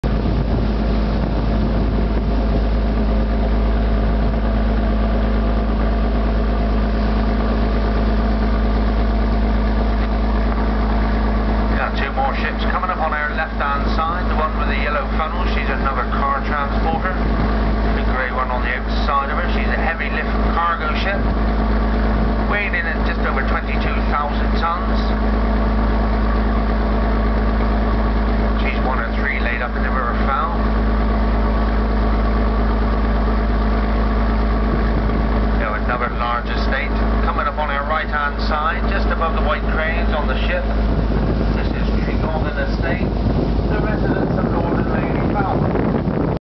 Laid Up Ships in the River Fal
A close-up view from a Fal river cruise from Falmouth